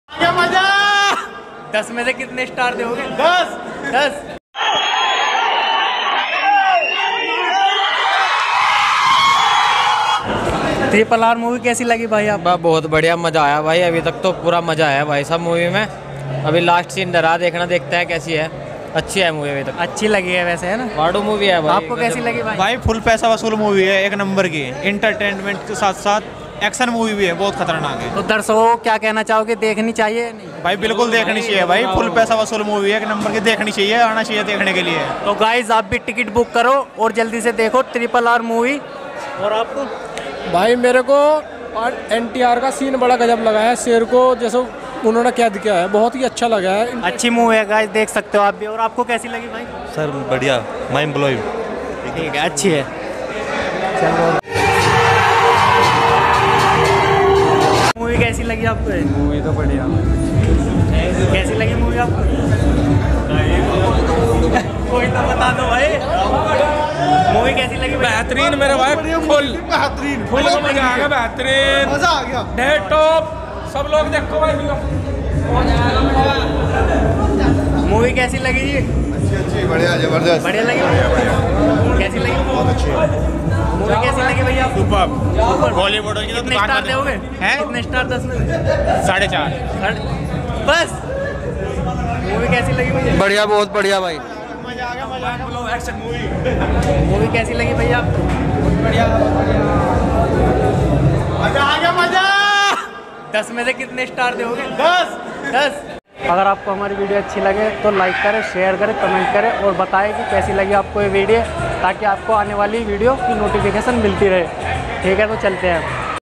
RRR Movie Public Review Hindi | 2nd Day 3rd Show | Public Talk | NTR, Ram Charan, SS Rajamouli
RRR Movie Public Review Hindi | 2nd Day 3rd Show | Public Talk | NTR, Ram Charan,

RRR movie public review hindi 2nd Day 3rd Show from gaiety galaxy theater Mumbai